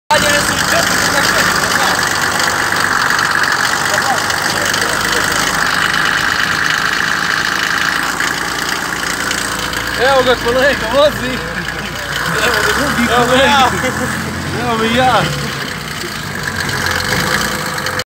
Uaz 469 Berane
Kolegijalna vožnja sa Ruskom mašinicom ;)